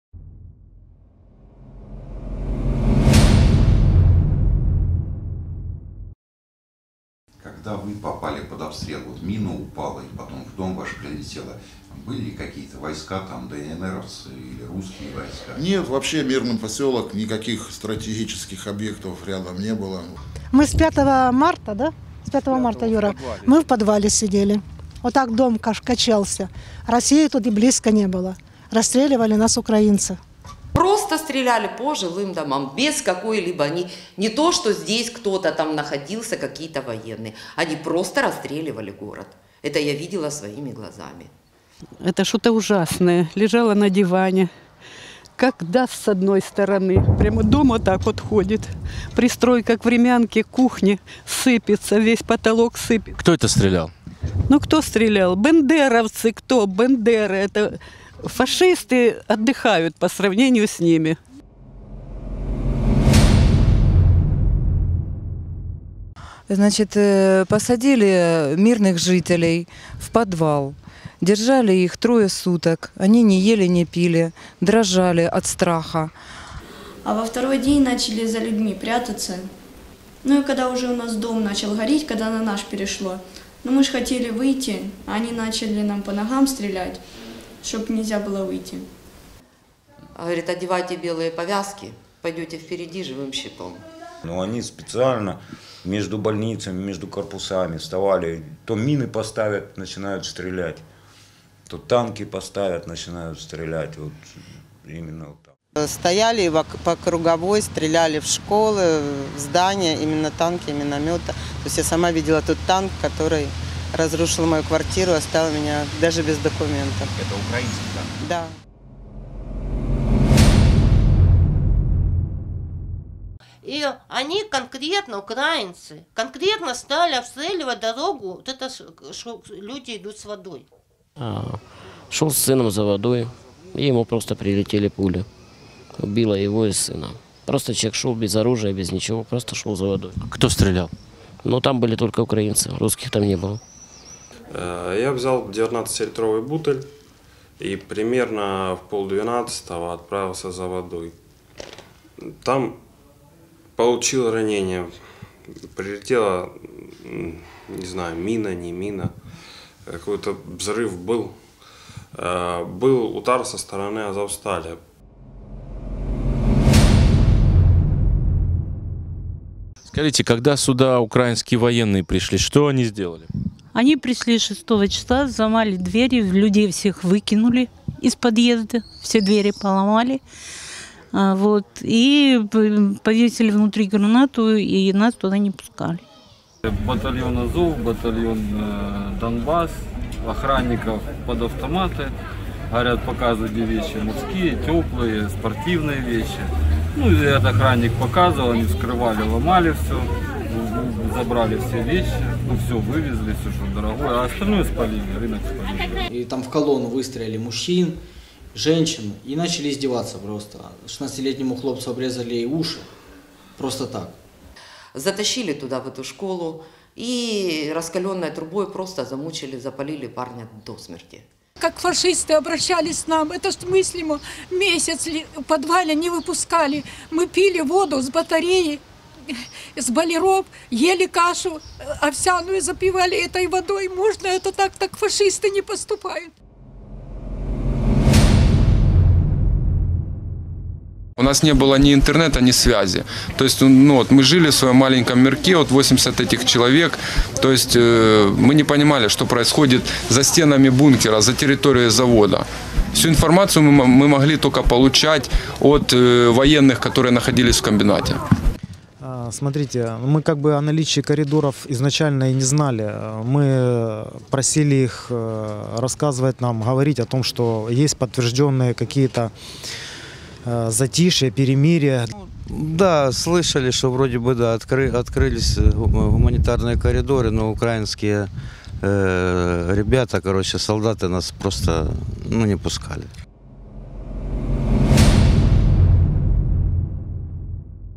MARIUPOL final statements of attrocities Witness accounts
Residents of Mariupol speak up - tell about the inhumane tactics of the Ukrainian Armed Forces, the atrocities of the Azov neo-Nazis & their attitude towards the civilian population. Witnesses interviewed by Maxim Grigoriev He presented this material at the conference at the Ukraine in the Public Chamber of Moscow, of which I and others attended.